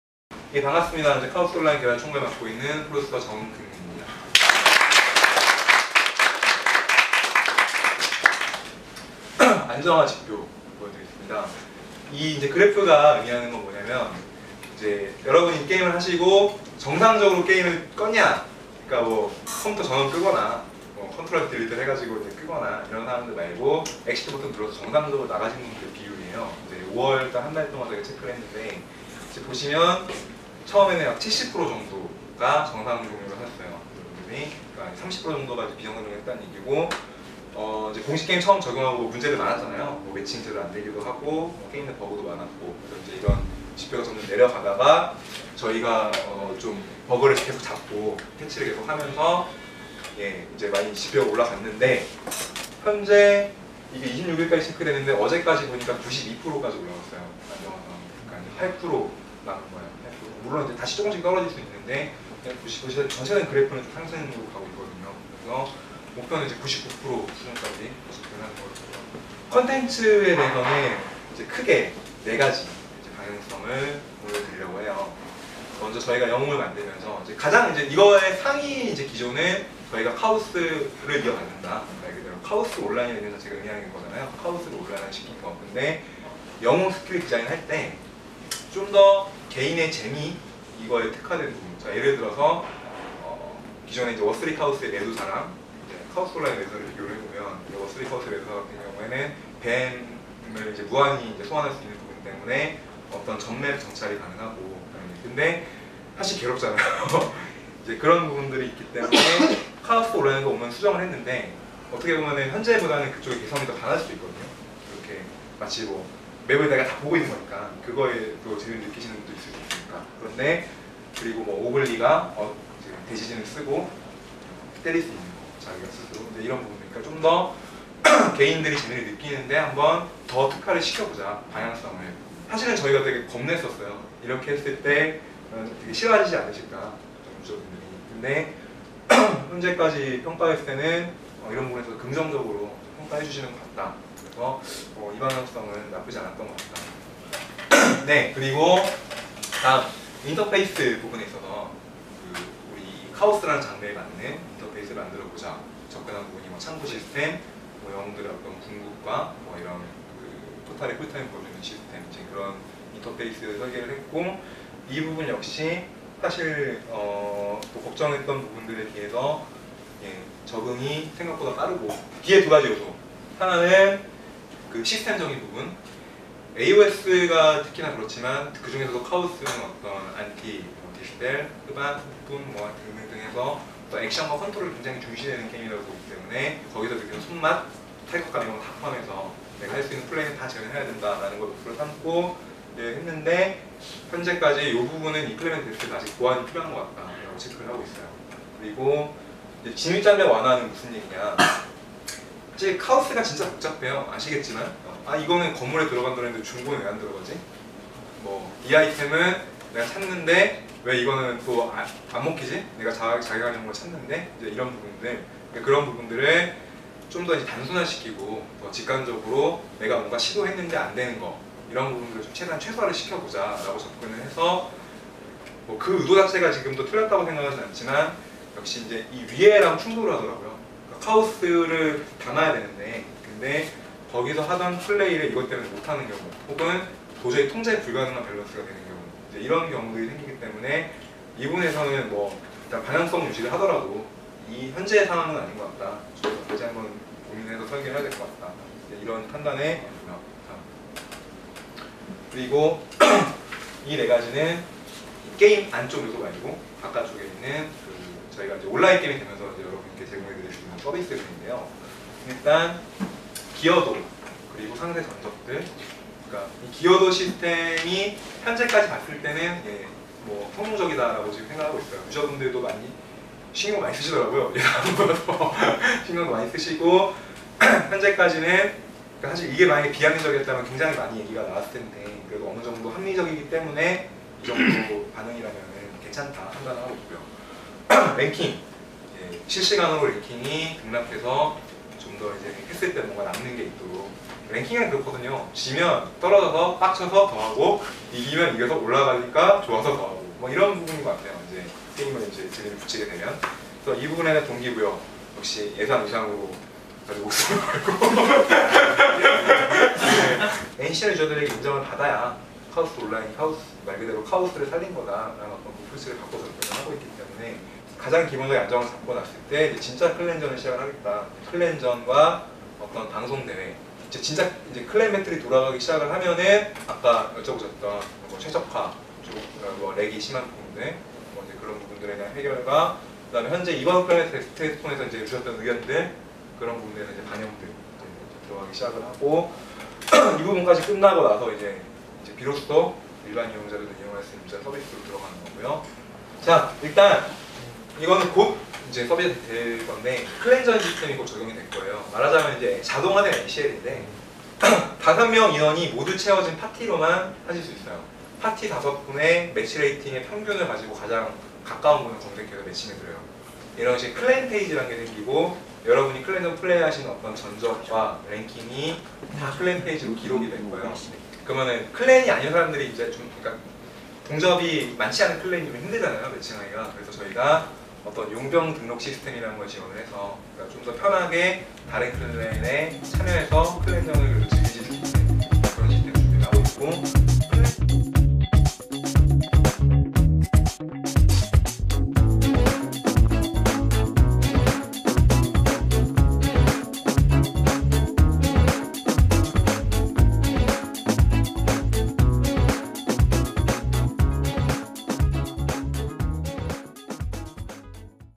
[카오스온라인] 클렌 배틀 후 유저 간담회 영상 [Chaos Online] Clan Battle Postmortem
클랜 배틀 테스트 종료 후, 참여해 주신 유저 여러분과 함께 했던 간담회 영상입니다. 클랜 배틀 테스트의 의의와 게임의 방향성, 그리고 향후의 계획까지 많은 이야기들을 나누었습니다.

네오액트(대표 김현민)가 개발하고 세시소프트(대표 강성욱)에서 퍼블리싱하는 온라인게임 '카오스온라인'의 클랜 배틀 테스트가 성공리에 종료됐다.
 '클랜 배틀 테스트'라는 명칭으로 클로즈베타 테스트를 시작한 '카오스온라인'은 AOS 장르의 특성상 각 영웅간의 밸런스와 시스템의 안정성이 중요한 요소이기에 5개월이라는 긴 기간동안 기존 카오스 클랜원들을 대상으로 밸런스 및 안정성에 대한 테스트를 실시했다. 이번 테스트에서는 44개 영웅이 등장해 밸런스를 맞췄으며, 안정성에 대해서도 만족할만한 결과를 얻었다는 평가다.
테스트에 참여했던 유저들은 '카오스온라인'이 '워크래프트3 카오스'의 느낌을 제대로 구현해 쉽게 익숙해질 수 있으면서도 기존 '워3 카오스'와 다른 점도 많아 새로운 게임을 즐기는 듯한 느낌이라는 의견과 더불어 오픈베타 시 하드웨어 사양만 낮춰준다면 기존 워3 카오스를 뛰어넘는 게임이 될 것이라고 기대감을 나타냈다.